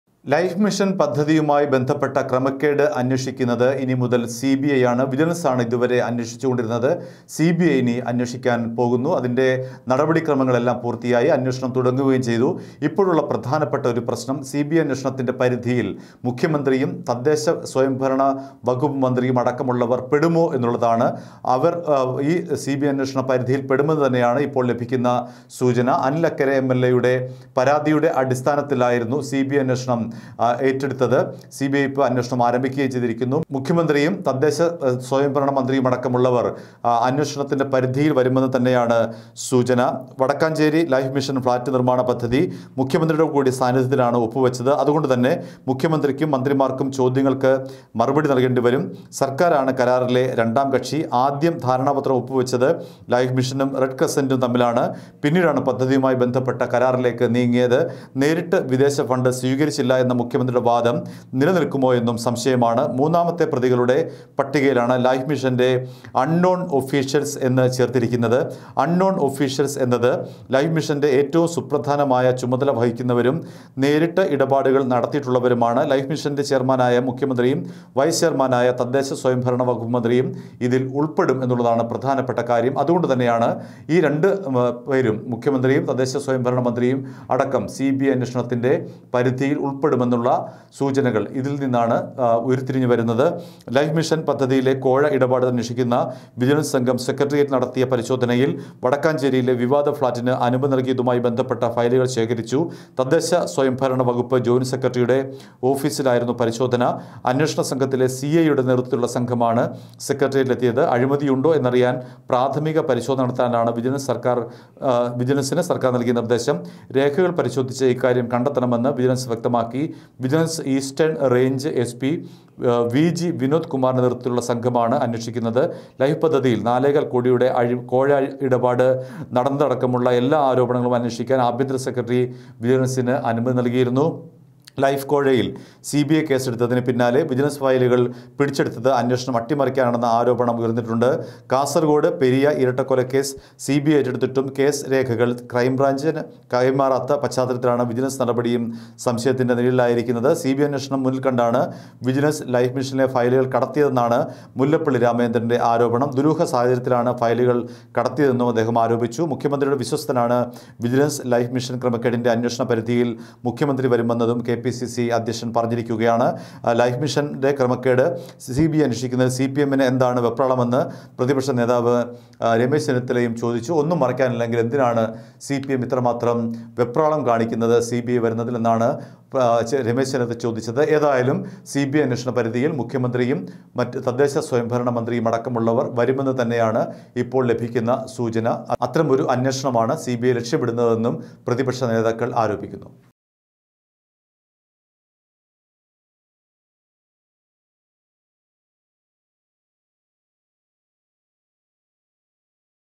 CBI ചോദ്യങ്ങള്‍ക്ക് മുഖ്യമന്ത്രിക്ക് മറുപടി പറയേണ്ടിവരുമോ ? | Life Mission Kerala CM
ലൈഫ്മിഷന്‍ ക്രമക്കേട് സിബിഐ ഏറ്റെടുത്തതോടെ മുഖ്യമന്ത്രിയും തദ്ദേശസ്വയംഭരണമന്ത്രിയും അന്വേഷണപരിധിയില്‍ വരുമെന്ന് സൂചന. വിജിലന്‍സ് ഫയലുകള്‍ പിടിച്ചെടുത്തത് സിബിഐ അന്വേഷണം അട്ടിമറിക്കാനെന്ന് പ്രതിപക്ഷം